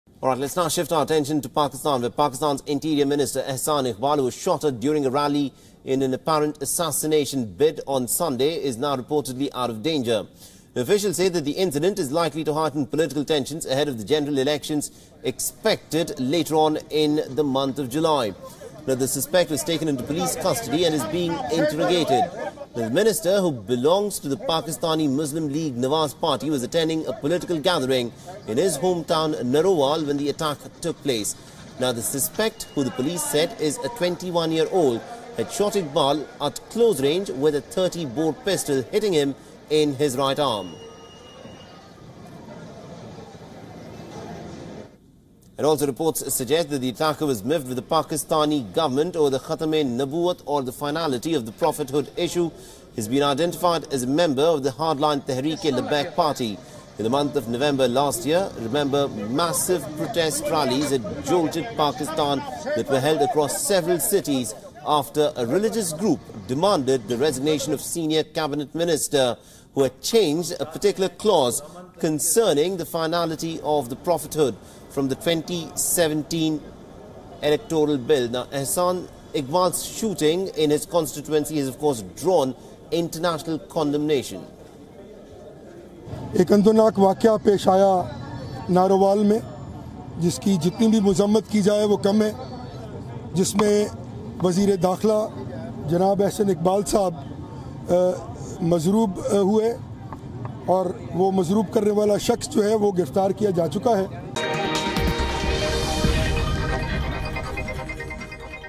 Pakistan's interior minister Ahsan Iqbal 'out of danger' after being shot at election rally
This segment of WION brings to you latest information on Pakistan's interior minister Ahsan Iqbal who is 'out of danger' after being shot at election rally. Watch this video for more info!